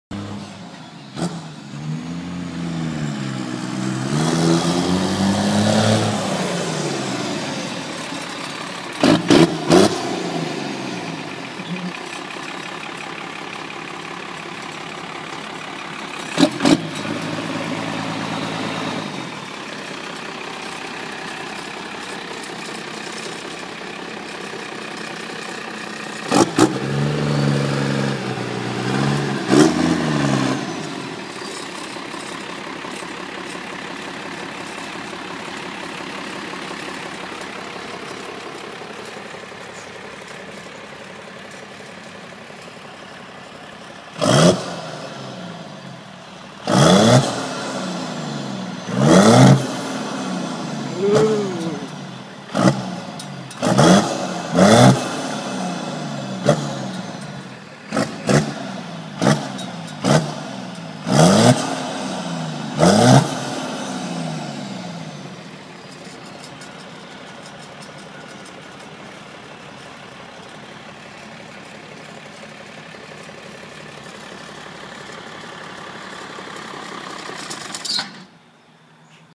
12 valve Cummins Rollin' Coal
1995 Dodge Ram 2500 Turbo Diesel Blowin Smoke